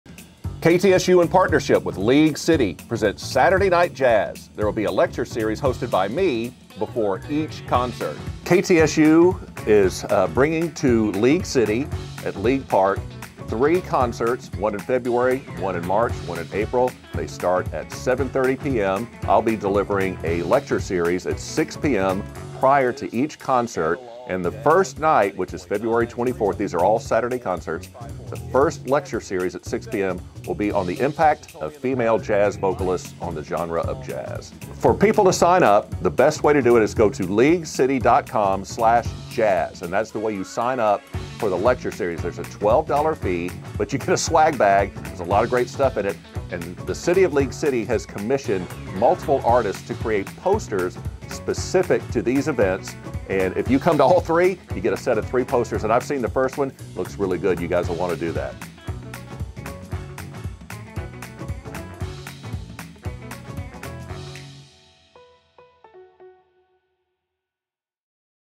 JAZZ LECTURE AND CONCERT FEB 24 🎷
The City is teaming up with KTSU-FM 90.9 and Galveston Bay Brewing to host a “Saturday Jazz Night” concert and lecture on February 24.
Before the FREE concert at 7:30 p.m. at League Park, KTSU radio personality Todd Parker will host a one-hour lecture from 6 to 7 p.m. The first lecture will focus on Female Jazz Artists.
The lecture costs $12, requires pre-registration, and will include a commemorative poster and swag bag.